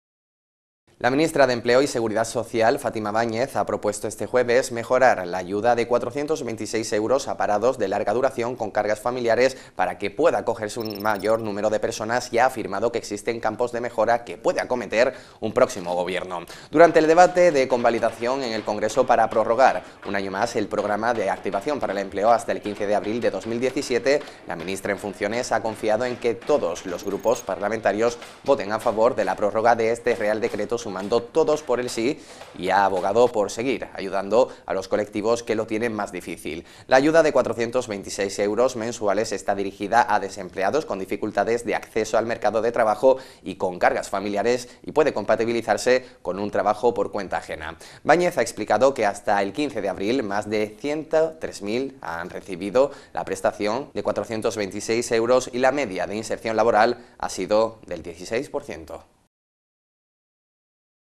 Málaga 24h TV - Báñez propone que el próximo Gobierno mejore la ayuda a parados
La ministra de Empleo y Seguridad Social, Fátima Báñez, ha propuesto este jueves mejorar la ayuda de 426 euros a parados de larga duración con cargas familiares para que pueda acogerse un mayor número de personas y ha afirmado que existen "campos de mejora que puede acometer un próximo Gobierno".

Durante el debate de convalidación en el Congreso para prorrogar un año más el Programa de Activación para el Empleo hasta el 15 de abril de 2017, la ministra en funciones ha confiado en que todos los grupos parlamentarios voten a favor de la prórroga de este Real Decreto sumando todos por el sí y ha abogado por seguir ayudando a los colectivos que lo tienen más difícil.

La ayuda de 426 euros mensuales está dirigida a desempleados con dificultades de acceso al mercado de trabajo y con cargas familiares y puede compatibilizarse con un trabajo por cuenta ajena.

Báñez ha explicado que hasta el 15 de abril más de 103.000 han recibido la prestación de 426 euros y la media de inserción laboral ha sido del 16%.

English: The Minister of Employment and Social Security, Fátima Báñez has proposed Thursday improve aid of EUR 426 long-term unemployed with dependents so that it can benefit a greater number of people and said that there are "areas of improvement that can be undertaken near Government ".

During the discussion of validation in Congress to extend for one year the Activation Program for Employment until April 15, 2017, the minister in office has confidence that all parliamentary groups to vote in favor of the extension of this Royal Decree adding all by itself and has advocated further assist groups that find it more difficult.

The aid of 426 euros per month is aimed at unemployed people with difficulties in accessing the labor market and family responsibilities and can be made compatible with paid employment.

Báñez explained that until April 15, more than 103,000 have received the benefit of 426 euros and the average employment was 16%.